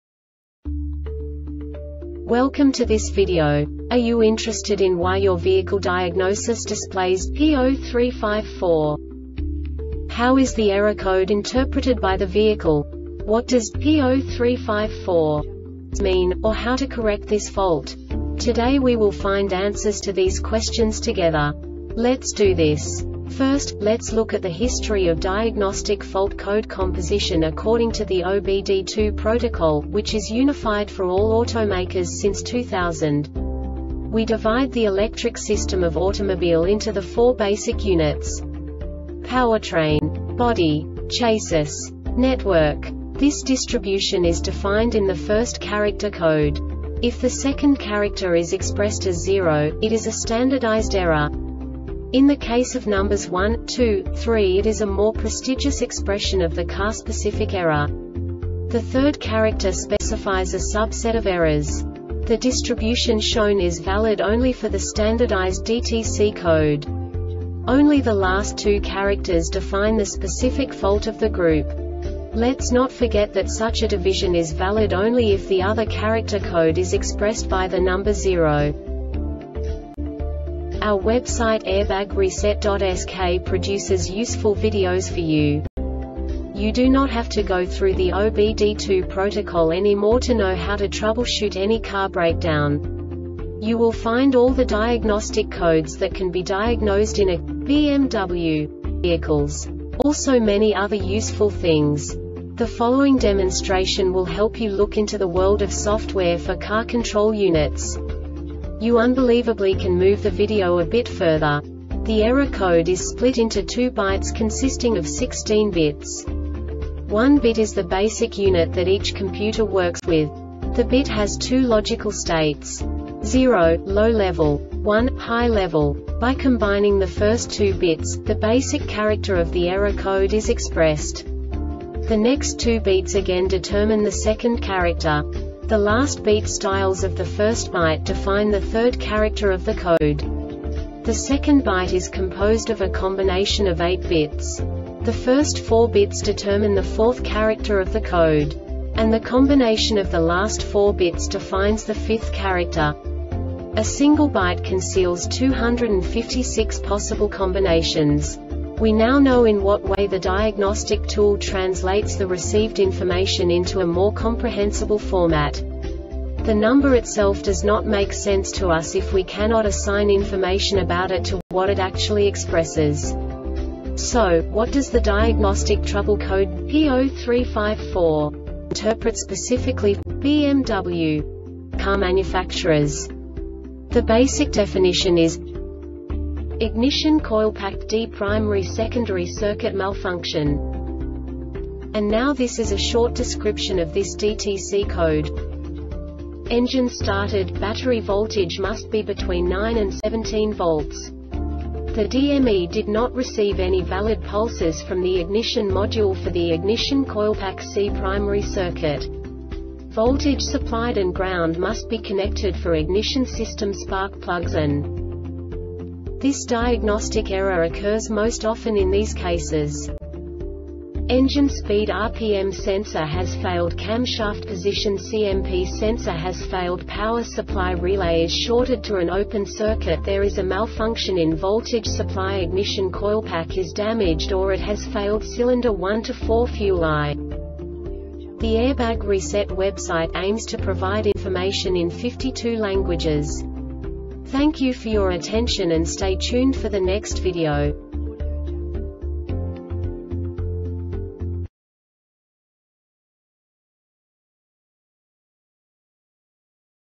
DTC BMW P0354 Short Explanation
Contents:
0:21 Basic DTC analysis according to OBD2 protocol standard.
1:48 Insight into programming
2:58 A diagnostically understandable description of the Diagnostic Trouble Code
3:05 Basic error definition

Make:
BMW
Code:
P0354
Definition:
Ignition Coilpack D Primary/Secondary Circuit Malfunction
Description:
Engine started, battery voltage must be between 9 and 17 volts. The DME did not receive any valid pulses from the ignition module for the Ignition Coilpack C primary circuit. Voltage supplied and ground must be connected for ignition system spark plugs an
Cause:
Engine speed (RPM) sensor has failed
Camshaft Position (CMP) sensor has failed
Power Supply Relay is shorted to an open circuit
There is a malfunction in voltage supply
Ignition coilpack is damaged or it has failed
Cylinder 1 to 4 Fuel I